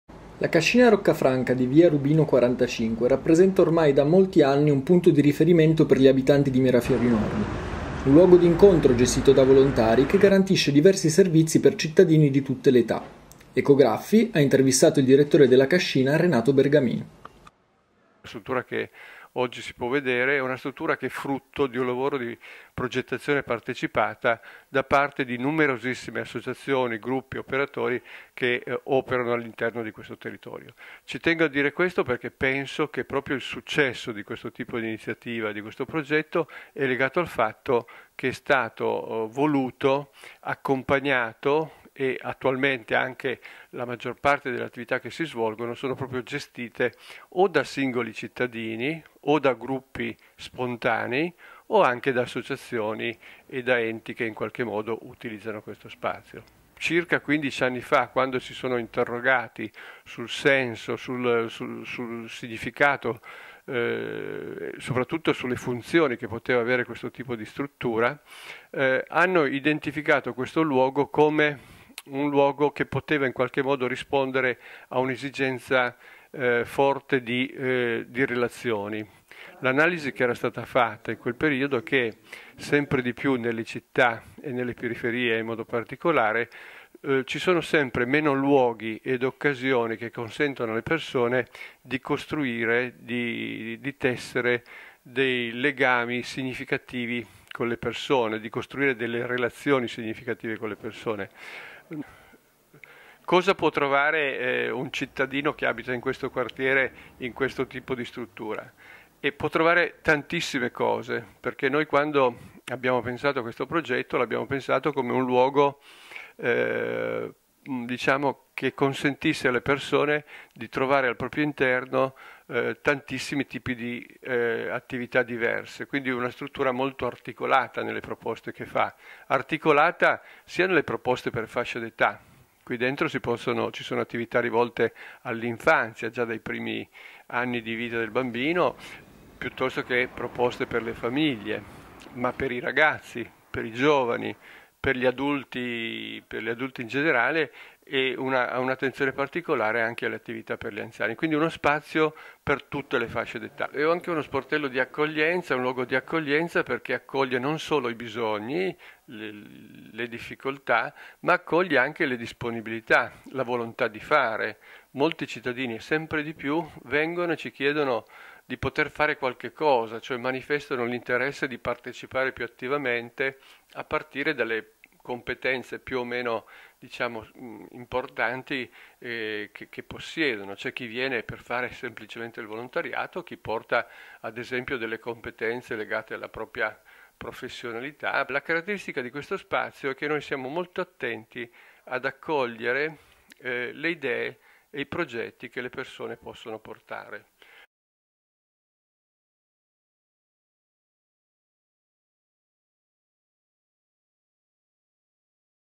La Cascina Roccafranca
UNA CASA PER IL QUARTIERE - Federico Cuomo ha intervistato il direttore di Cascina Roccafranca Renato Bergamin: storia e iniziative di un centro polifunzionale che ha saputo trasformare Mirafiori Nord